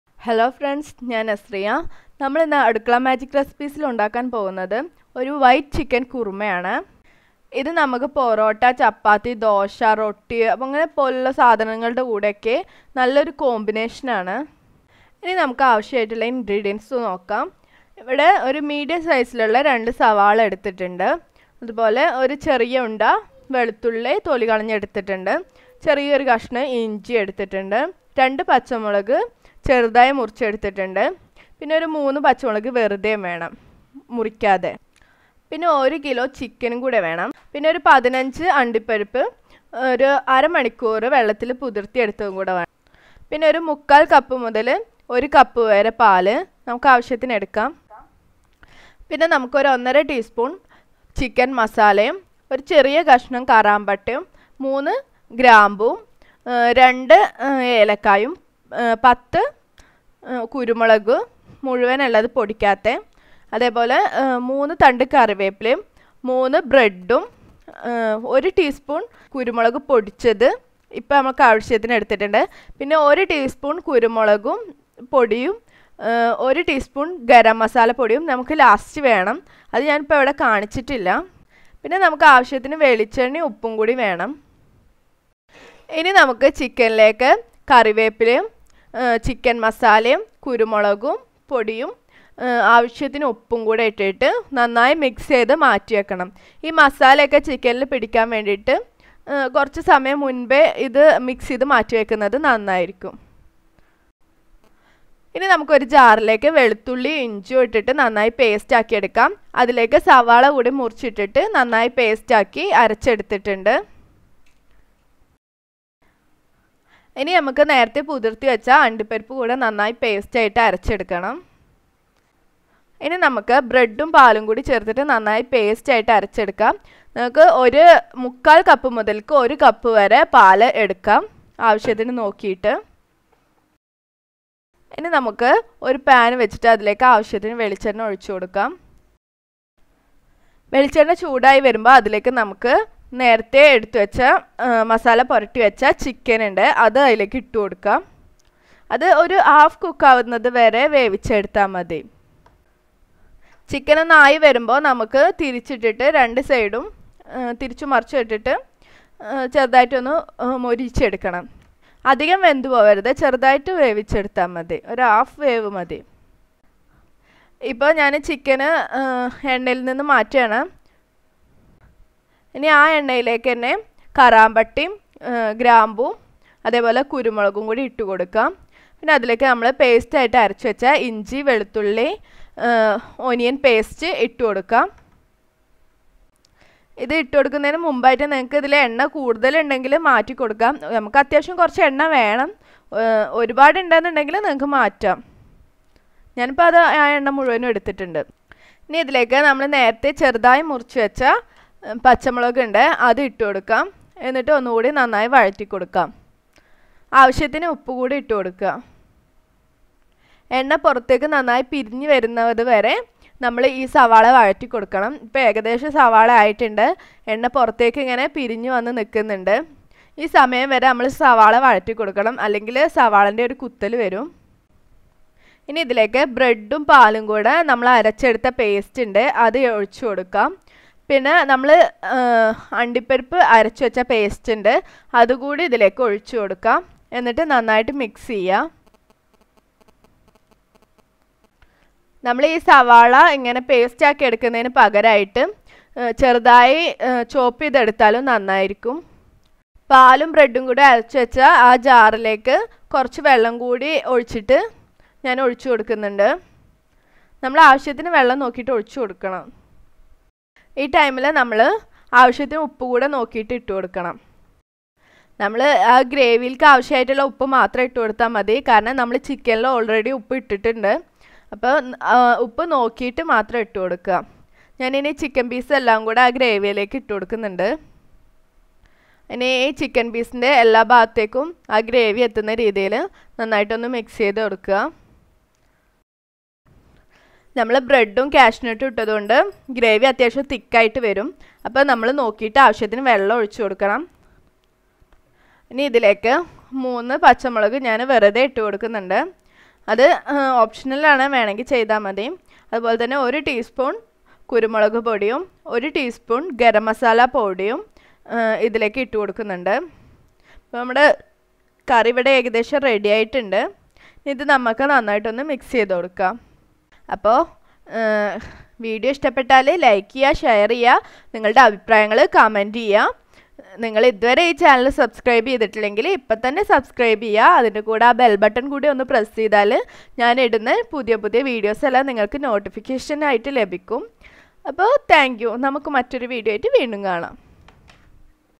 എളുപ്പത്തിൽ ഉണ്ടാക്കാവുന്ന വൈറ്റ് ചിക്കൻ കുറുമ | White Chicken Korma | White chicken curry/Kurma
പൊറോട്ട , ചപ്പാത്തി , ദോശ , പൂരി , റൊട്ടി തുടങ്ങിയവയുടെ കൂടെ കഴിക്കാൻ പറ്റിയ അടിപൊളി കുറുമ ആണ് വൈറ്റ് ചിക്കൻ കുറുമ.
 ചിക്കനു പകരം ബീഫ് അല്ലെങ്കിൽ മട്ടൻ വേണമെകിൽ ഉപയോഗിക്കാം .

kerala porotta yude koodeyum , chappati , dosa , rotti thudangiyavaude koodeyum nalla combination ulla  adiploi korma/curri aanu white chicken korma.